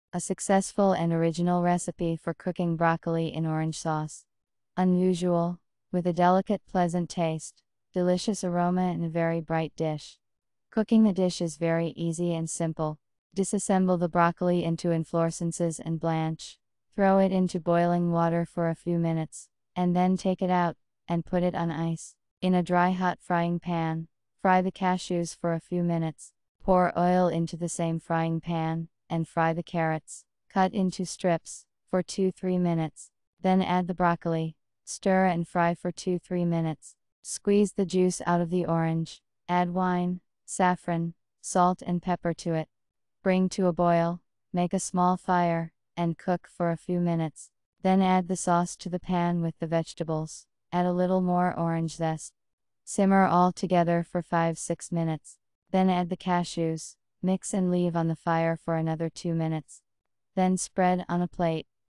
Broccoli in orange sauce
A successful and original recipe for cooking broccoli in orange sauce. Unusual, with a delicate pleasant taste, delicious aroma and a very bright dish. Cooking a dish is very easy and simple!

Ingredients:
Broccoli — 300 Gram,
Orange— 2 Pieces,
Dry white wine — 150 Milliliters,
Saffron — 1 Pinch,
Cashews — 50 Gram,
Salt — 1-2 Pinches (to taste),
Ground black pepper — 1 Pinch (to taste),